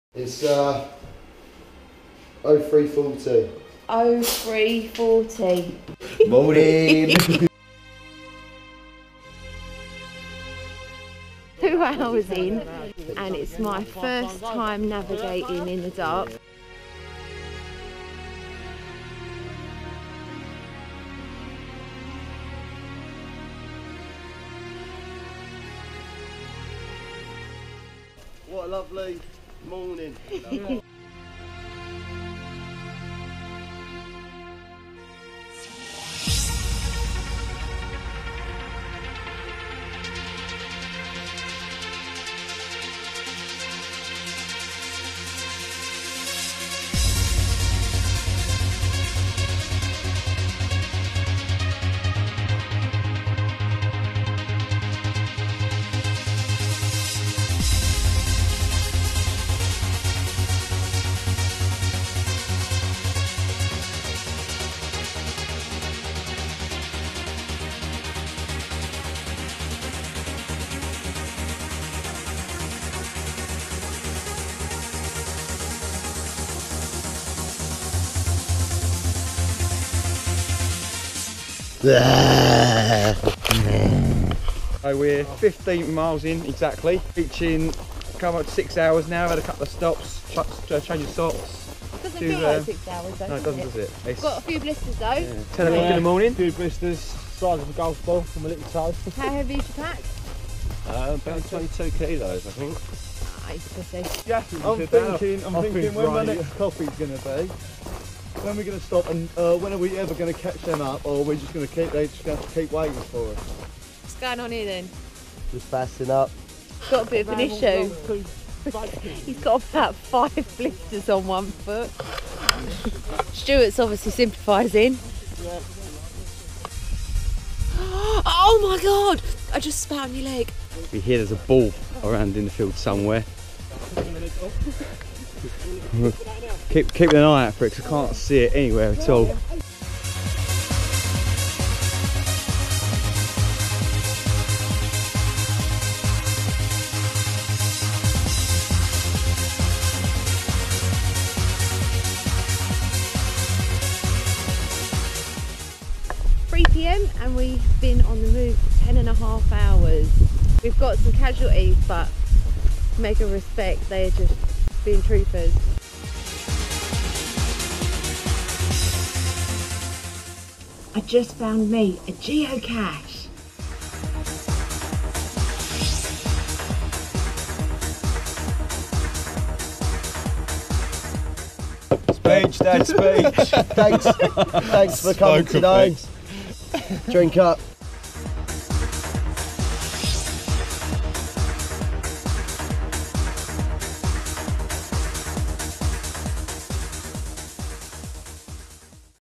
The Essex Way Hike
On Sunday 23rd October 2016, myself and 7 friends hit up the Essex Way fora  fun day out.

We set off at 04:30 with the plan to cover 40 miles, but after getting a little lots in the dark, some rum and then finding a pub, we turned the miles into kilometres.

A fantastic day with fantastic people xxxxx